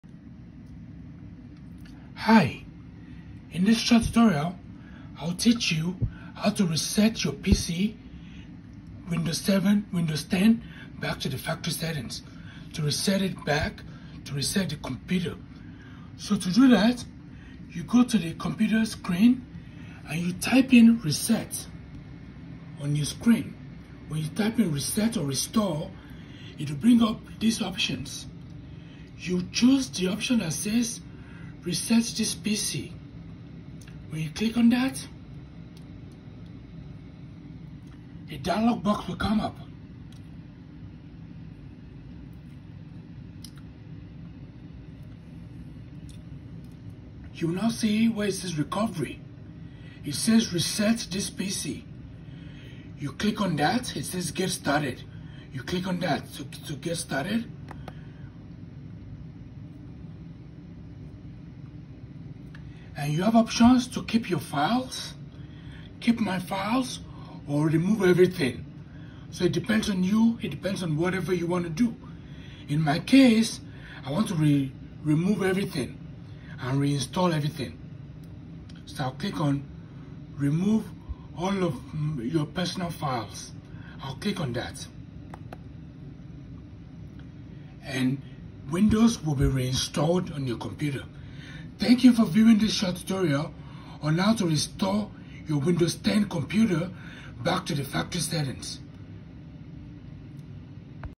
How to reinstall windows 10 on a computer to factory settings
A short tutorial on how to reinstall windows 10 on your computer